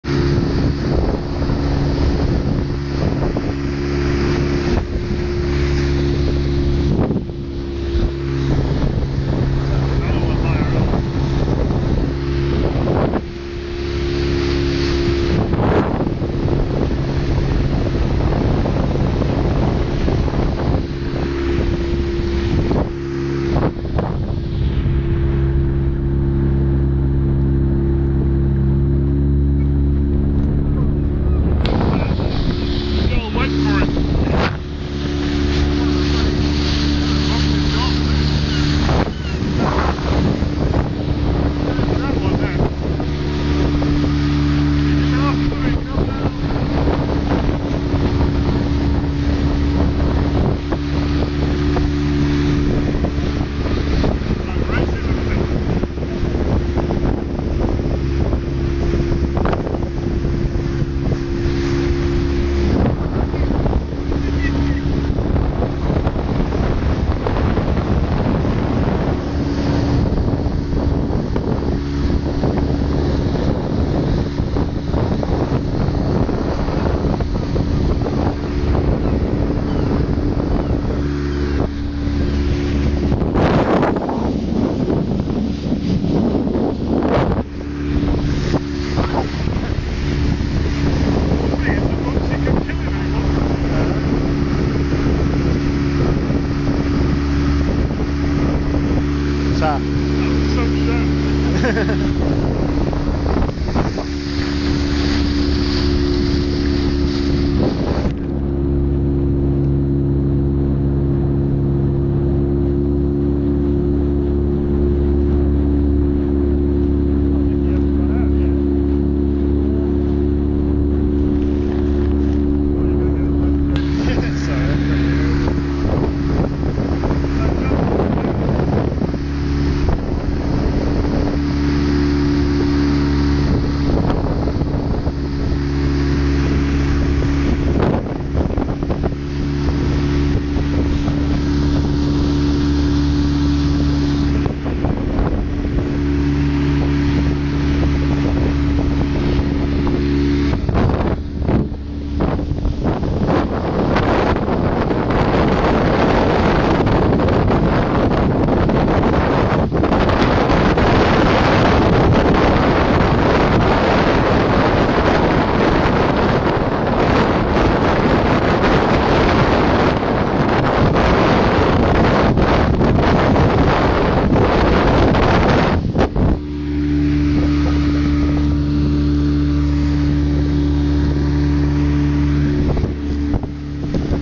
Fishing Trip
Coming back from a fishing trip in Orkney. Bonxie starts to follow us and is then attacked by seagulls!